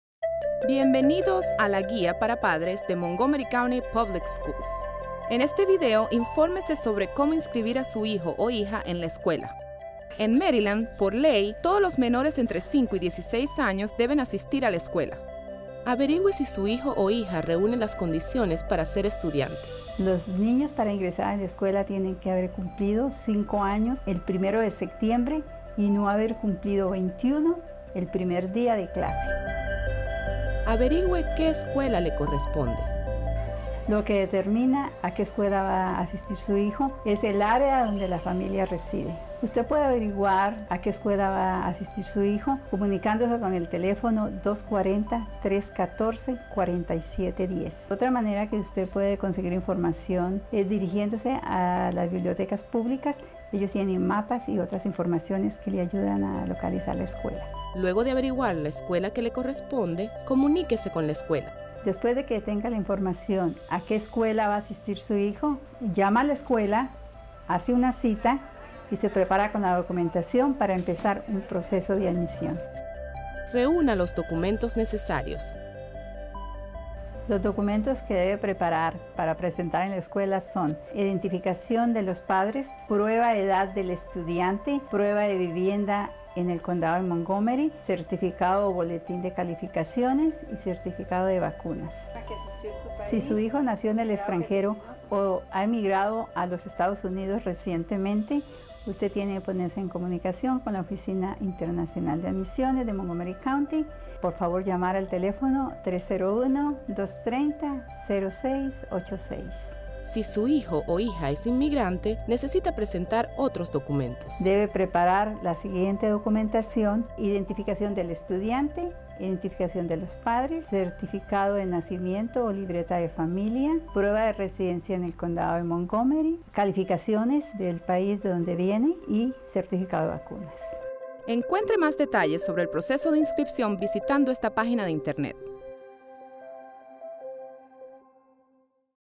¿Cómo inscribir a su hijo o hija en MCPS?
¿Es un nuevo residente el Condado de Montgomery? Este video le informará sobre los pasos a seguir para inscribir a su hijo en las Escuelas Públicas del Condado de Montgomery.